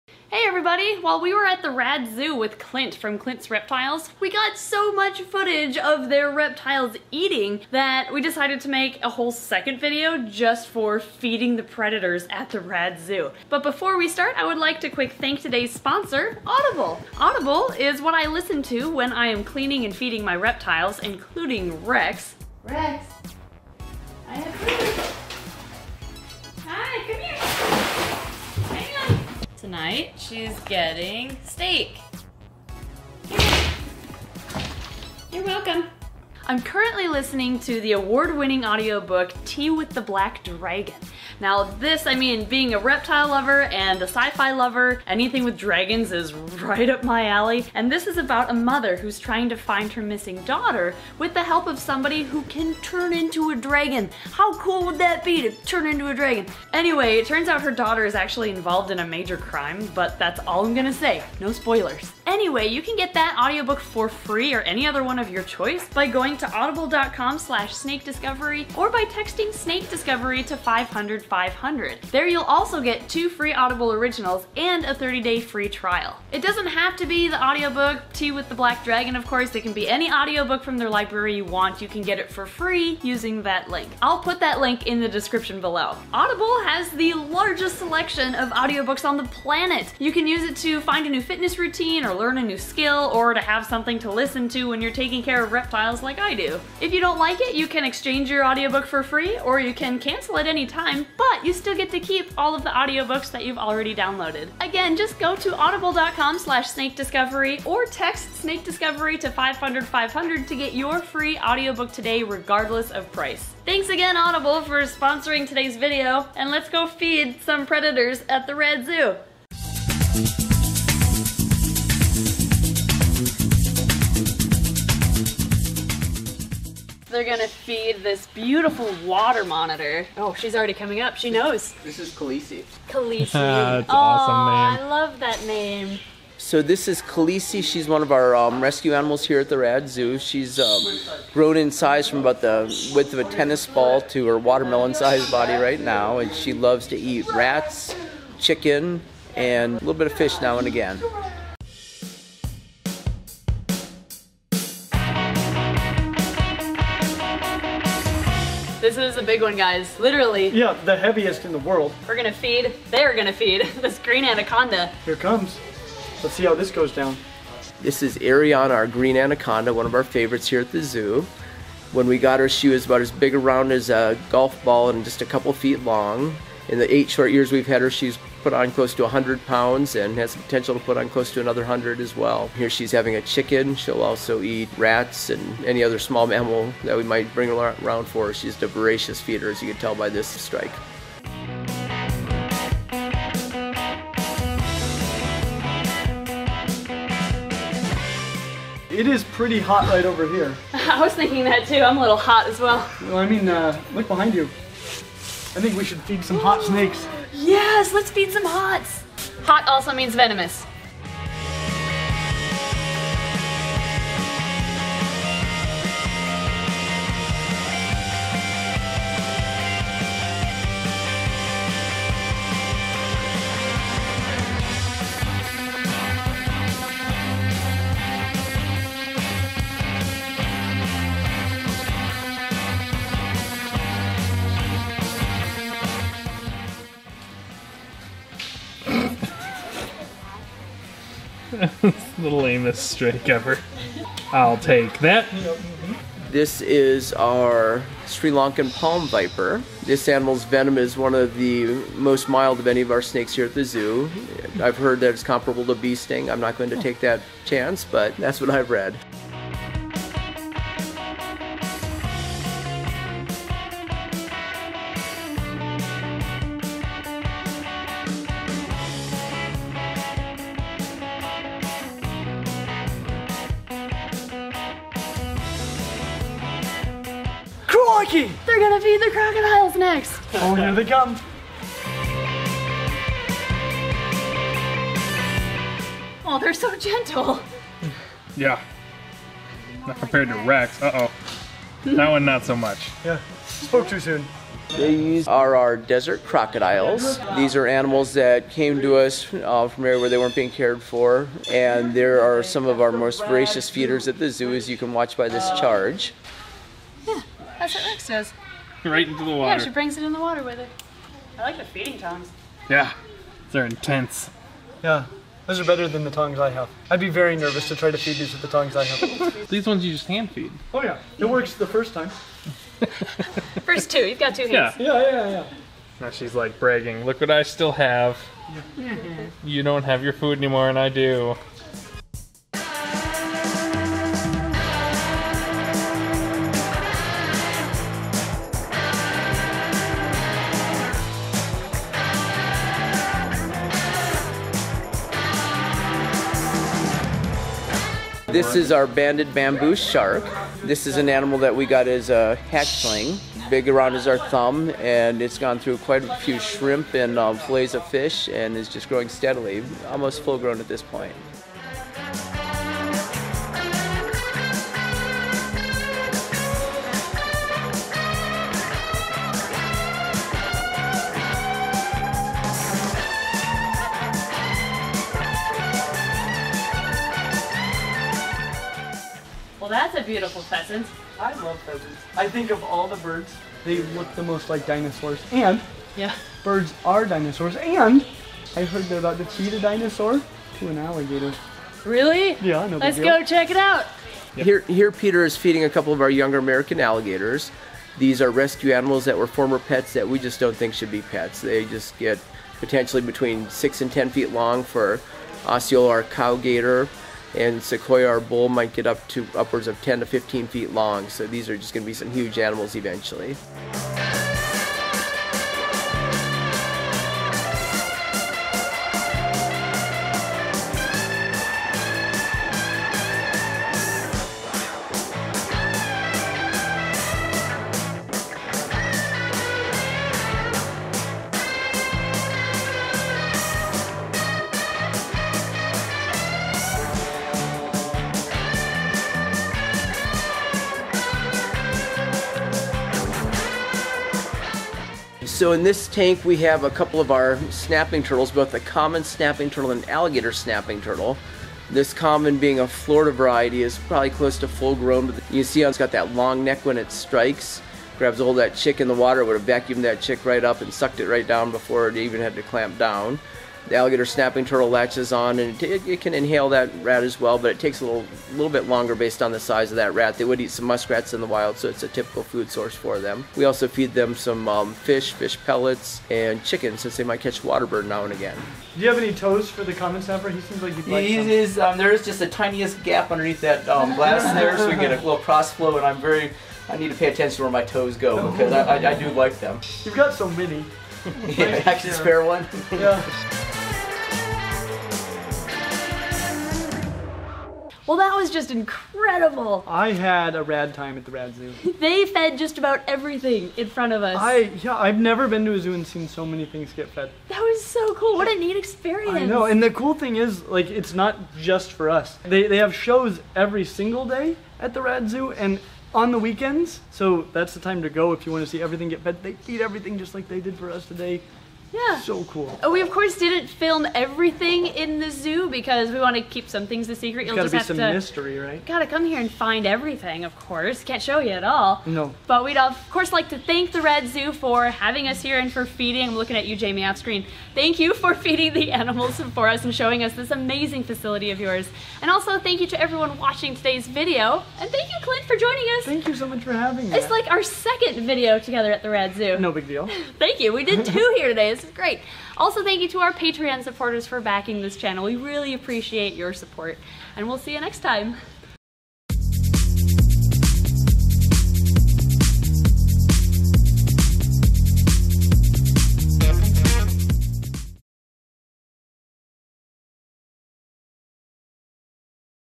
Feeding the PREDATORS at the RAD Zoo! (with Clint's Reptiles!)
or text SNAKEDISCOVERY to

We had so much awesome footage from our tour at the RAD Zoo that we decided to make a second video just for it! They fed their crocodiles, copperheads, vipers, monitors, and even their shark in front of us!! Enjoy!

Haven't heard of Clint? He has a fantastic YouTube focused on reptile education that you can find here!

We love fan mail!